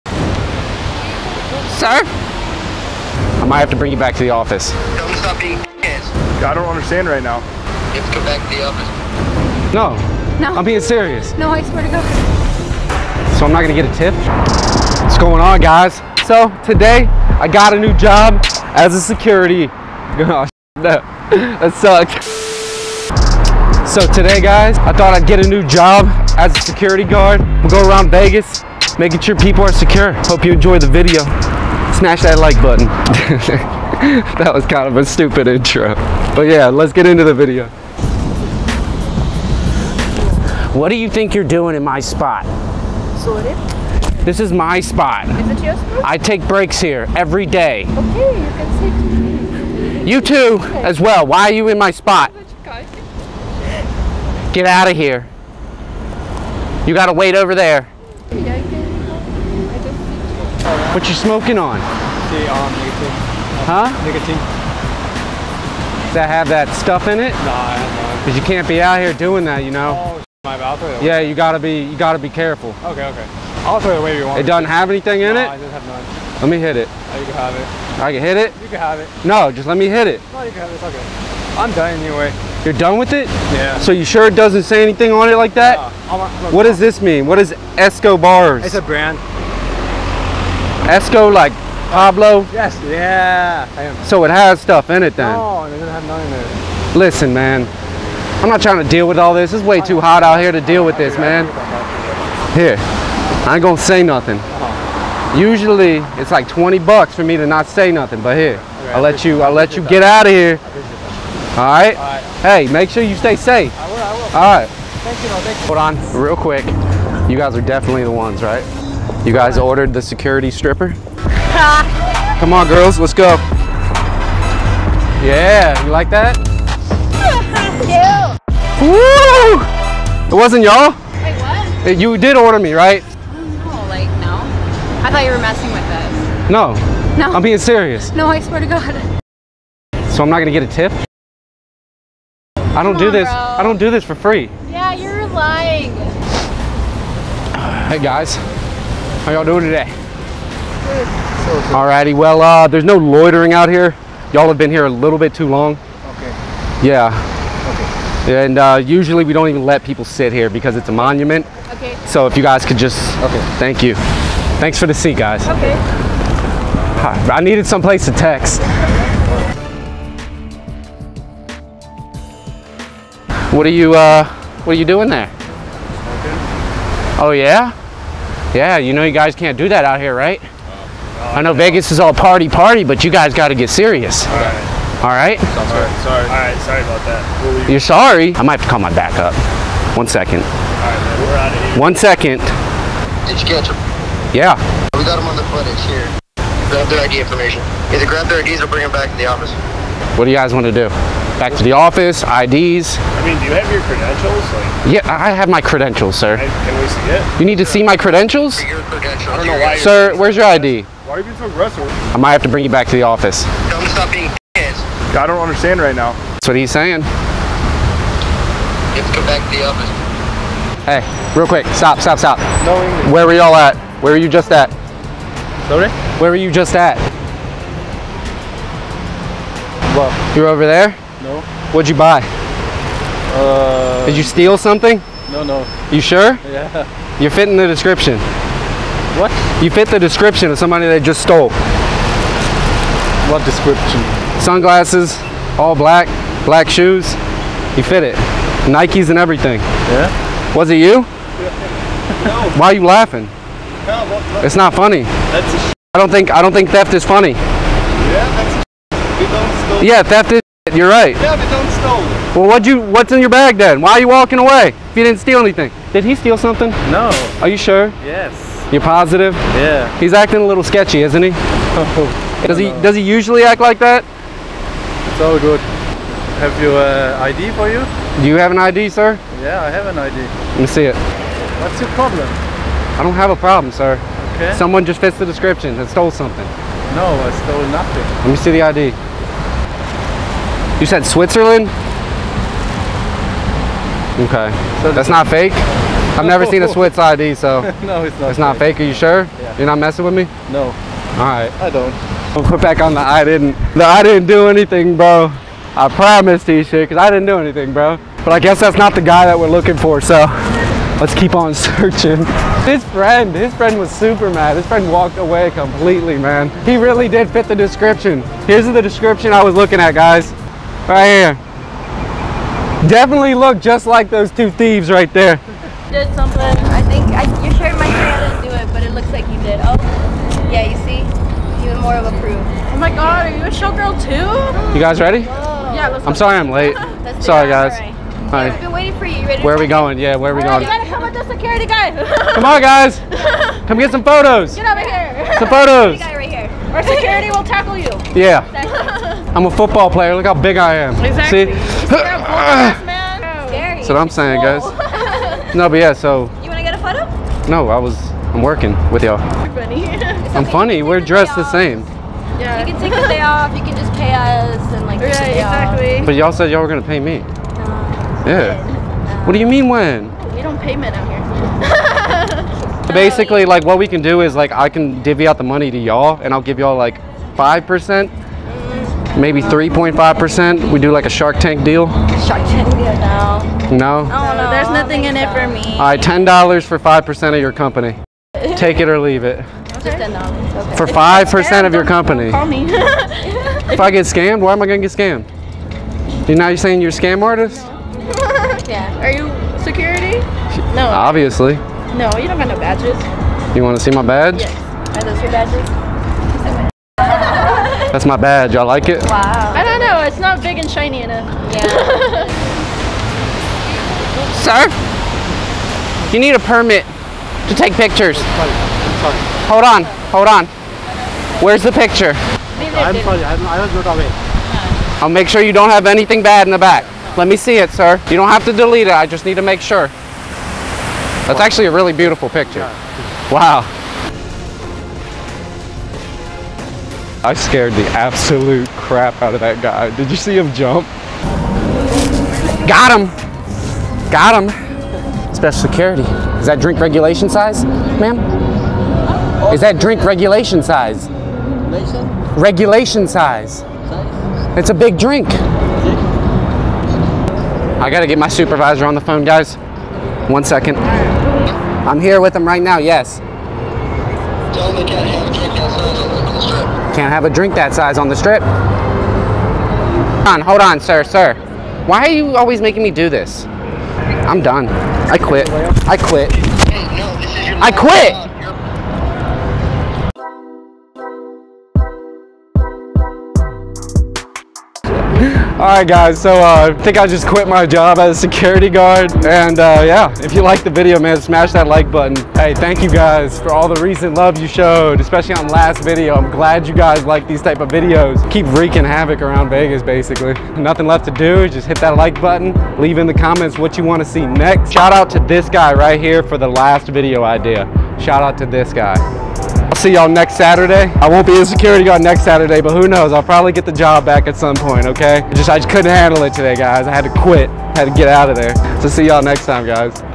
Security Guard Prank (LAS VEGAS)
😂 Another funny video this time we did the security guard prank.